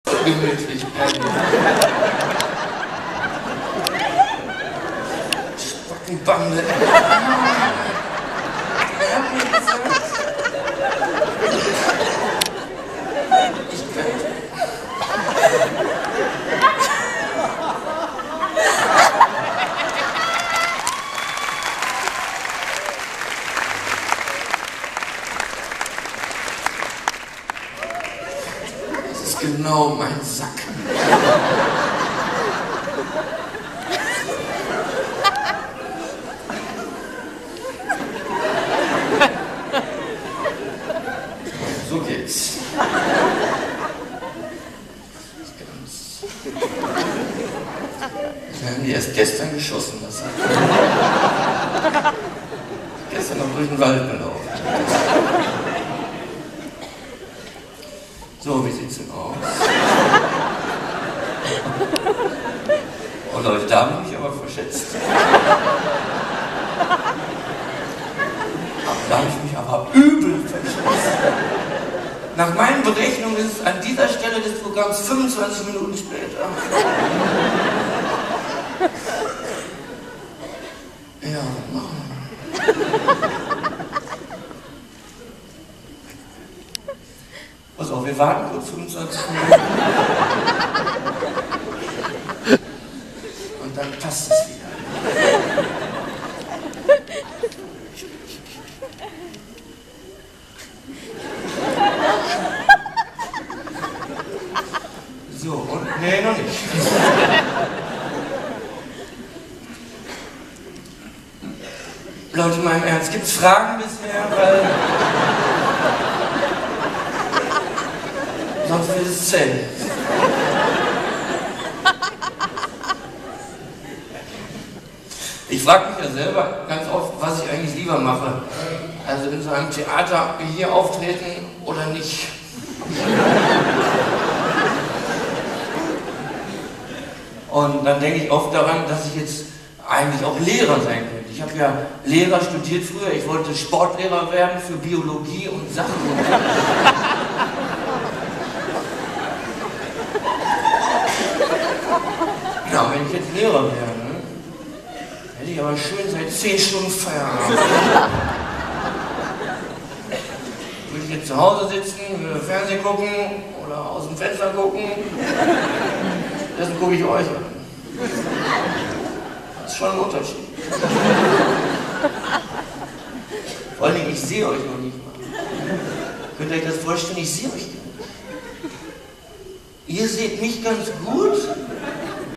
Johann König in Dresden 9.März 2013
Eine kleine Szene von Johann König im "alten Schlachthof" in Dresden

Feuer im Haus ist teuer, geh raus!

Der letzte Satz wurde leider nicht komplett aufgenommen.
Es war zum Schluss: "Ihr seht mich ganz gut...aber ich seh Euch nicht. Und das ist gut so!"